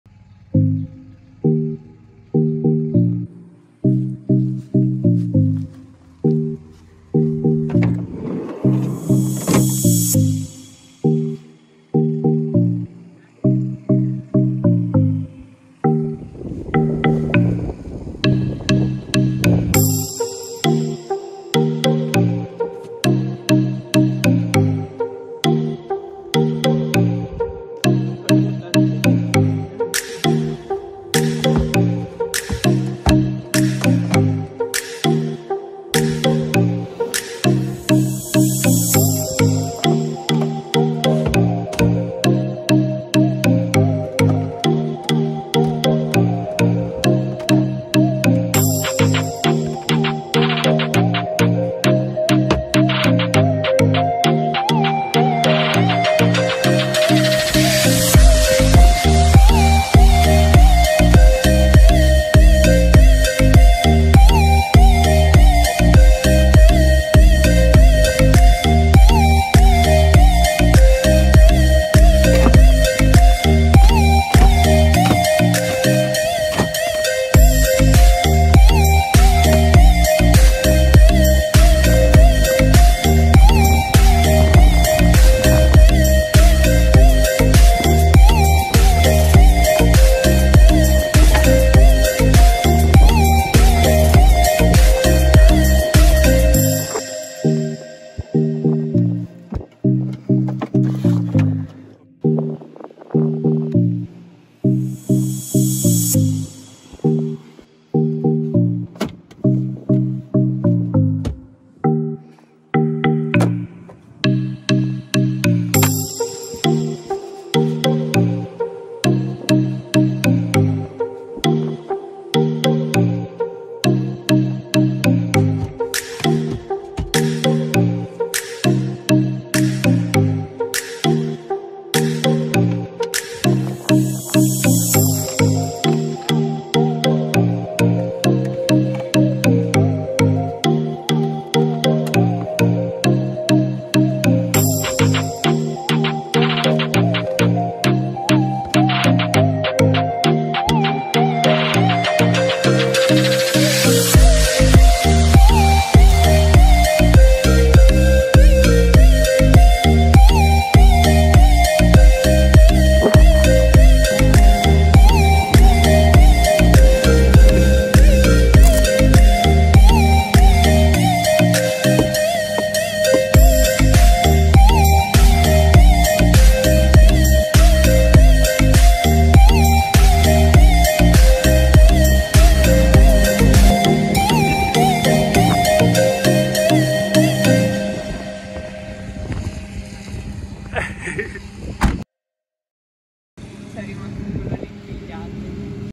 WV Caddy camper conversion • from 7 SEATS to DOUBLE BAD
Here's the self made conversion of my VW Caddy Maxi into a micro camper in only 10 minutes.

The design of the wood modules to has followed a single important rule: do not modify any internal part of the van.
I can basically switch the Caddy to the factory setting at anytime.